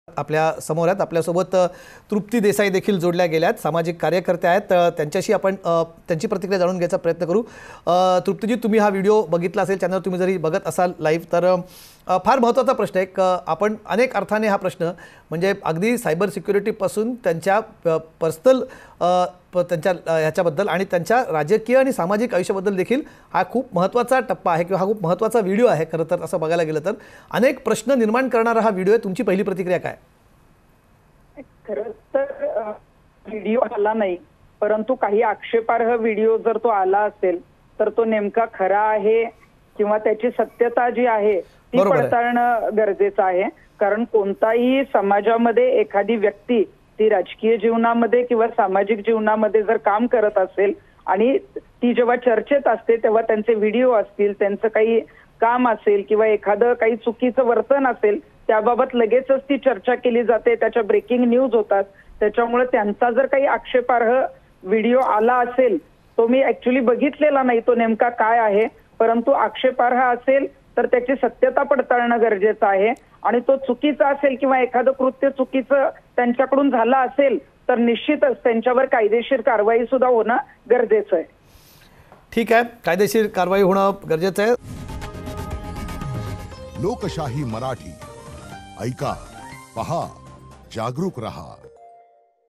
Trupti Desai On Kirit Somaiya MMS : तृप्ती देसाईची किरीट सोमय्यांच्या व्हिडिओवर प्रतिक्रिया
Sensational video of senior BJP leader Kirit Somaiya has reached the hands of democracy. In this video, Somaiya is seen in an offensive position. Due to the virality of this video, there has been a lot of excitement in the political circle. If the private videos of a big person like Kirit Somaiya are going viral, what about the general public? Such a question is being raised.

 भाजपचे वरिष्ठ नेते किरीट सोमय्या यांचा खळबळजनक व्हिडीओ लोकशाहीच्या हाती लागला आहे. या व्हिडीओत आक्षेपार्ह स्थितीत सोमय्या दिसत आहेत. हा व्हिडीओ व्हायरल झाल्यामुळे राजकीय वर्तुळात मोठी खळबळ उडाली आहे. किरीट सोमय्या यांच्यासारख्या मोठ्या व्यक्तीचे खासगी व्हिडीओ व्हायरल होत असतील तर सामान्य जनतेचे काय? असा प्रश्न उपस्थित केला जात आहे.